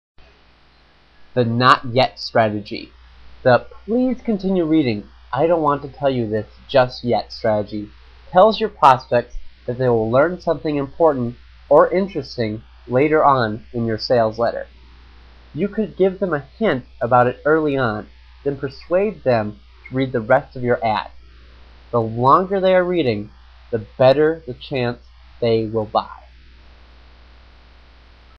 The "Not Yet" Strategy
The "please continue reading, I don't want to tell you just yet..." strategy tells your prospects that they will learn something important or interesting later on in your sales letter. You could give them a hint about it early on then persuade them to read the rest of your ad. The longer they are reading, the better the chance they will buy.